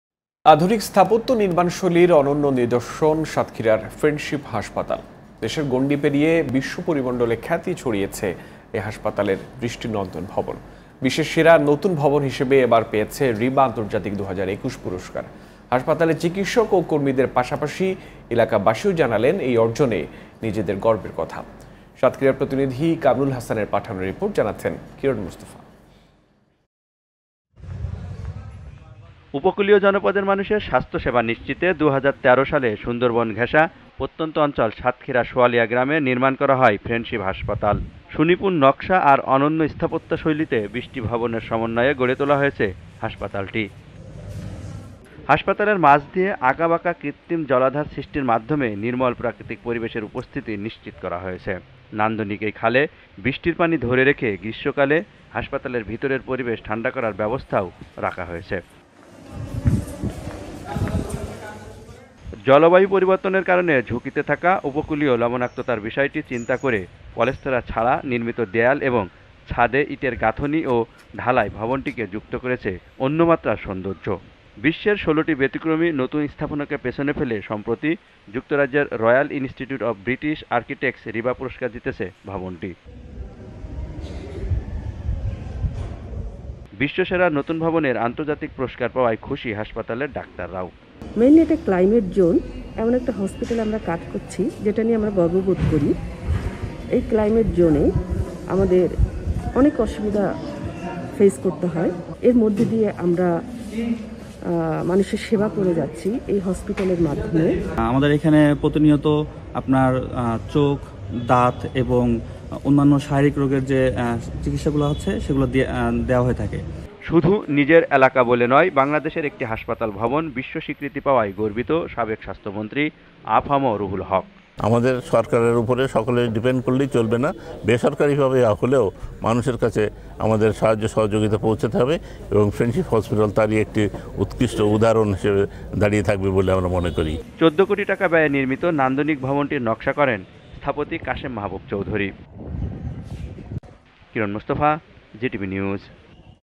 বিশ্বসেরা ভবনের পুরস্কার পেল সাতক্ষীরার ফ্রেন্ডশিপ হাসপাতাল | Friendship Hospital Satkhira
বিশ্বসেরা ভবনের পুরস্কার পেল সাতক্ষীরার ফ্রেন্ডশিপ হাসপাতাল | Friendship Hospital Satkhira | 03Jan22